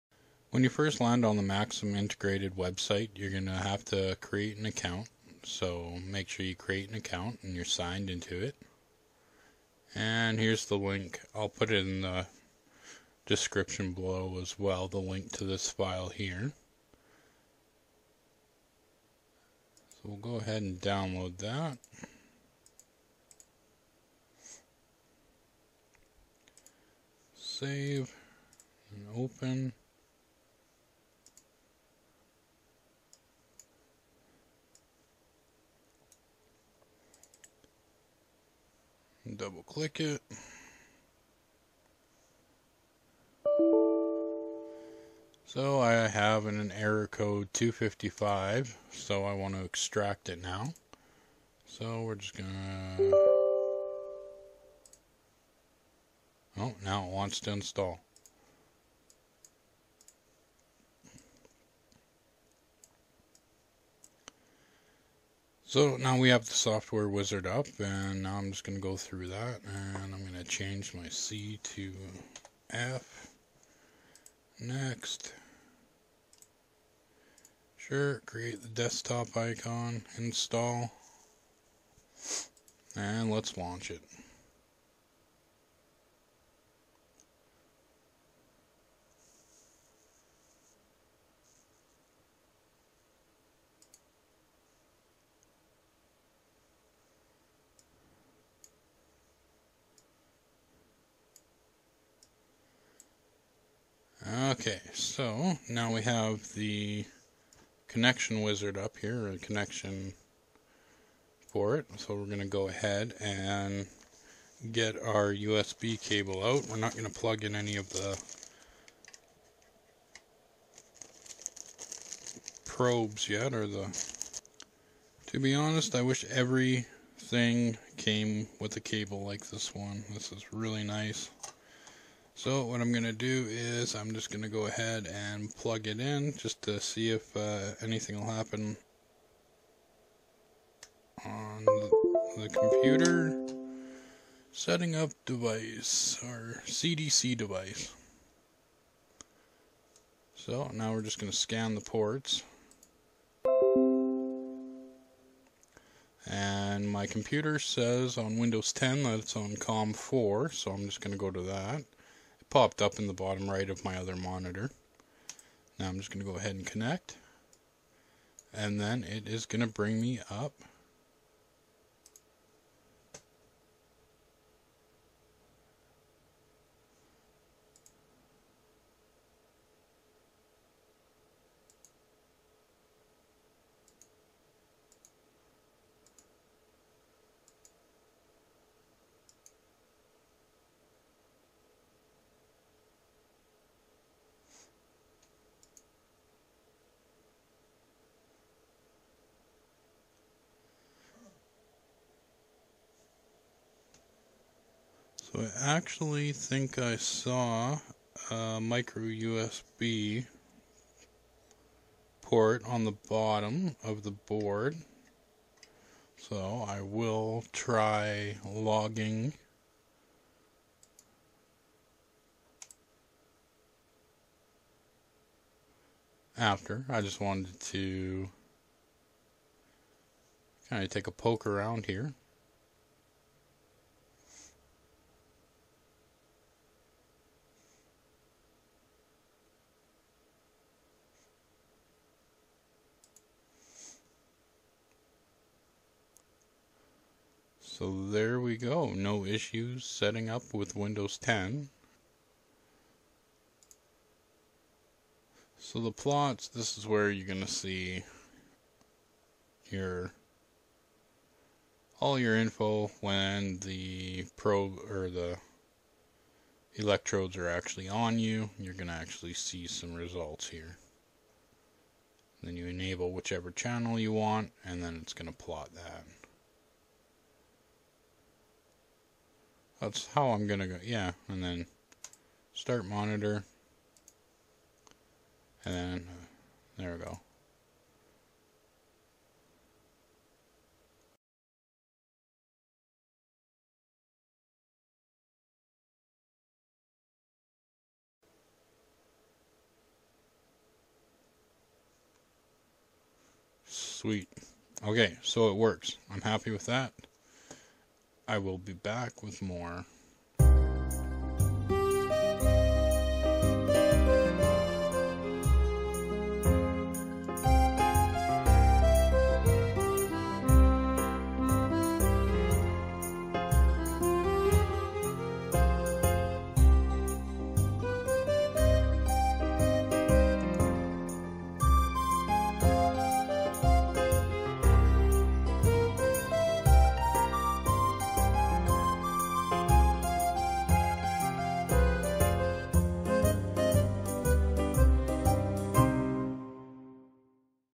Max30001 EVKIT Windows 10 Setup (ECG, BioZ, Heart Rate, and Pace)
I also setup on windows 7, after a reboot it seemed to work but from what I have seen there has been a few people having driver issues.  If you are having issues with the drivers reboot your computer and try to connect.  I did however seem to get random error in windows 7 over 10.  To me windows 10 would be your ideal environment for running the software in.